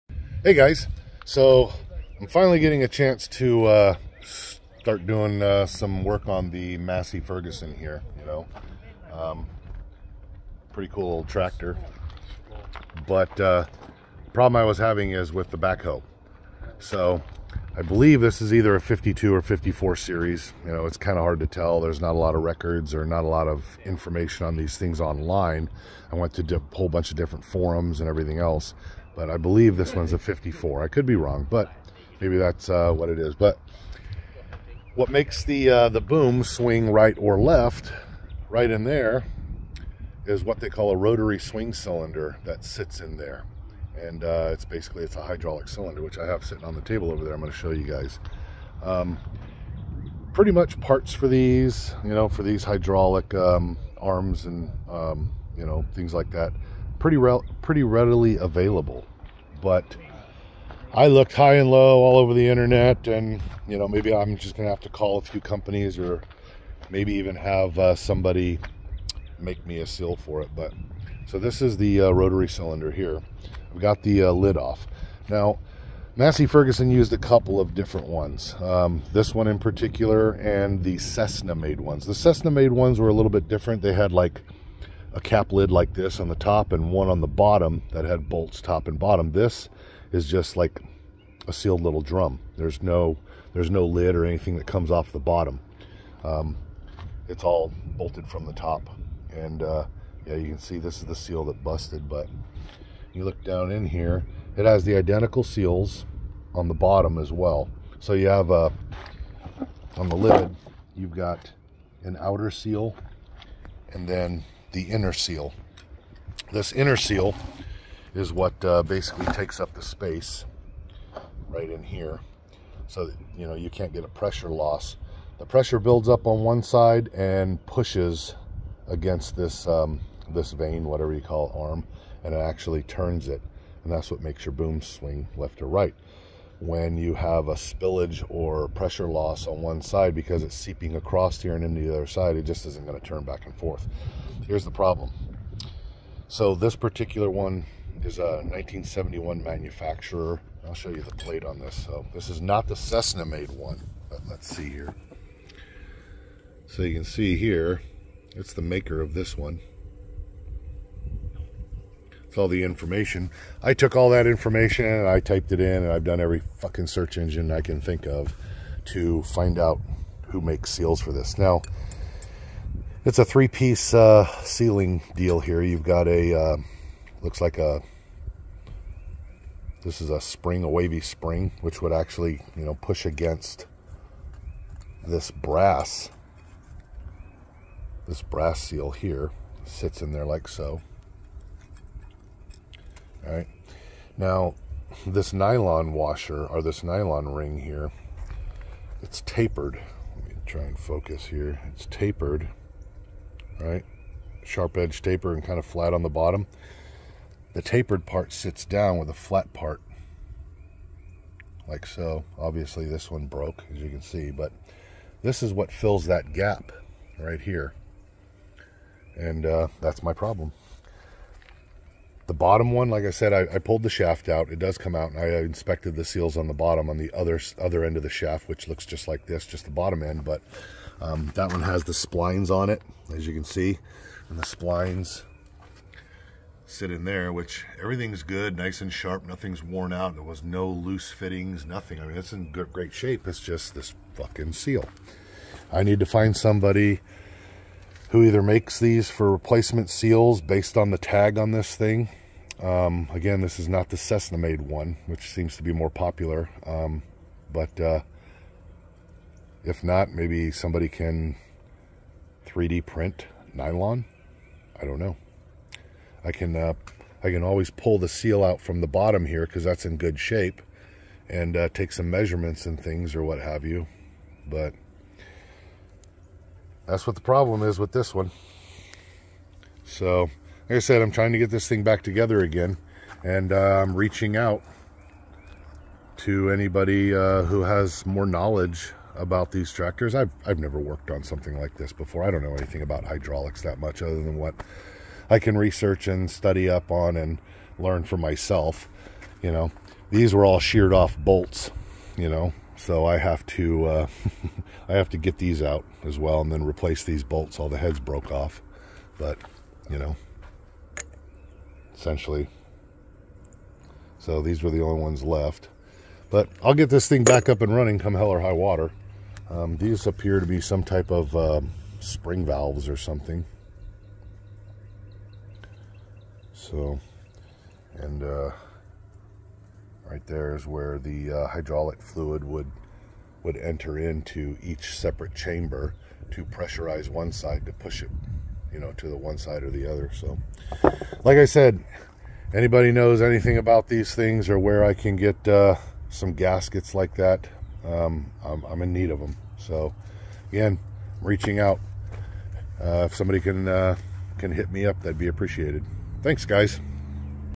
Help! 1971 Massey Ferguson hydraulic Rotary Swing Cylinder on an MF54 Backhoe has broken seal!
I have a Massey Ferguson 203 tractor with a MF54(?) Backhoe that came equipped with a Houdaille Industries made (Not Cessna) Hydraulic Rotary Swing Cylinder for Massey Ferguson. I need the inner tapered Nylon seal for it! If anyone knows where I can get that replacement seal(s) for it, please hit me up and point me in the right direction! I've searched high and low on the Web and came up with nothing! Can that Nylon seal be copied and 3D printed maybe? Thanks guys!